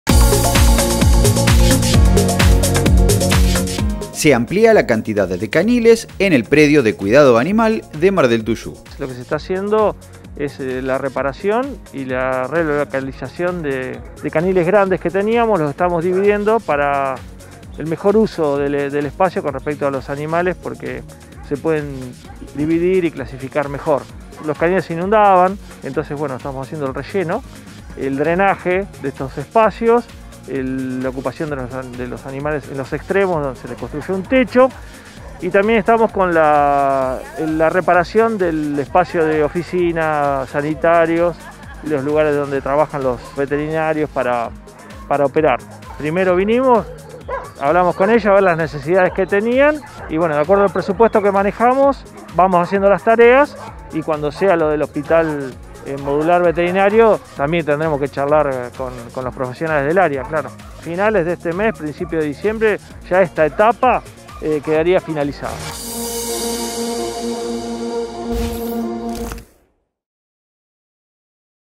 Se amplía a la capacidad del refugio animal
Se construyen nuevos caniles en el predio de cuidado animal de la municipalidad en Mar del Tuyú.